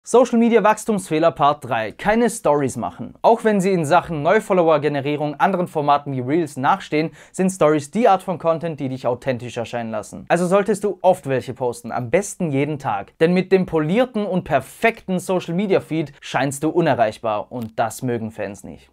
Warum Instagram Stories für Künstler:innen EXTREM wichtig sind!
Instagram Stories können deinen Erfolg auf Social Media Plattformen immens beeinflussen! 🎼